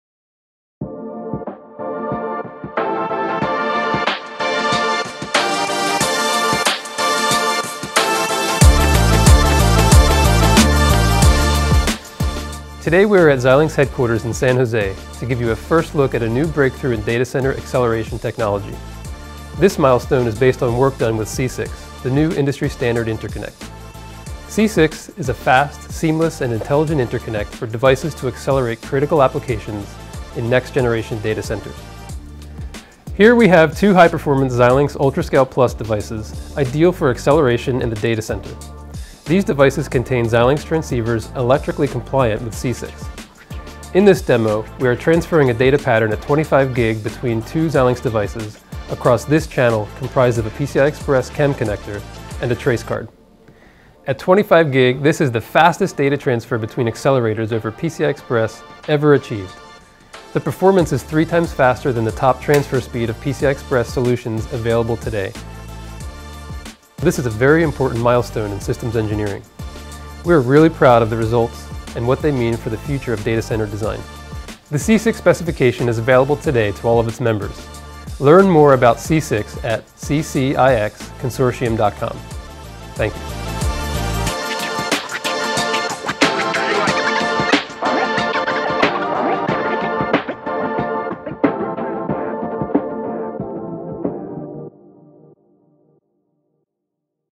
CCIX Demo Shows 25Gbps Performance over PCIe
In this demo video, see a data pattern transferred at 25 Gbps between two Xilinx FPGAs, across a channel comprised of an Amphenol/FCI PCI Express CEM connector and a trace card. The two FPGAs contain Xilinx transceivers electrically compliant with CCIX. By using the PCI Express infrastructure common today in all data centers, the 25 Gbps performance milestone was achieved. This is the fastest known data transfer between accelerators over PCI Express connections ever achieved and 3x faster than PCI Express Gen3 solutions available today.  For more information on the CCIX standard, go